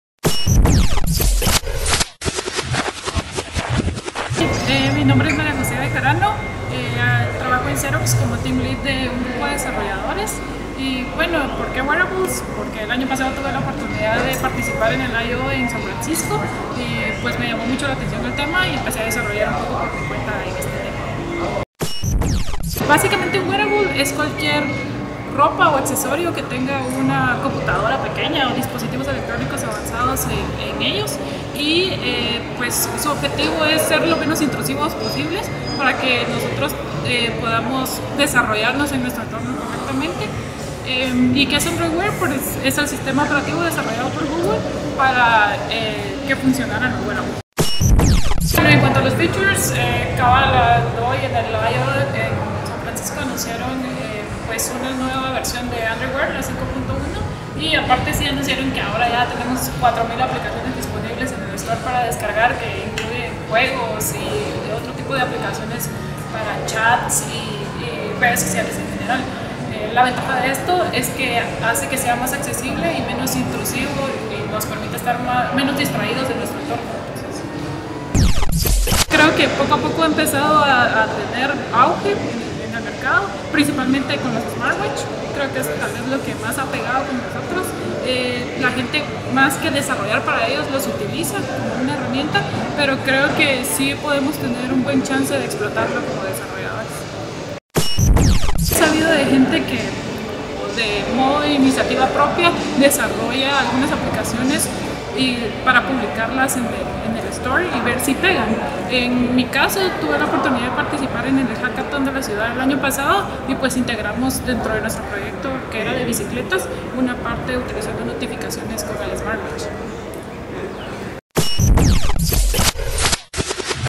GoogleIOExtendedGT - Los wearables de María José Bejarano
María José Bejarano nos cuenta de la puesta en escena de los wearables y Android Wear en Guatemala.

Fotografía: Santiago Billy
Edición: Miguel Ortiz
Diario Digital (2015).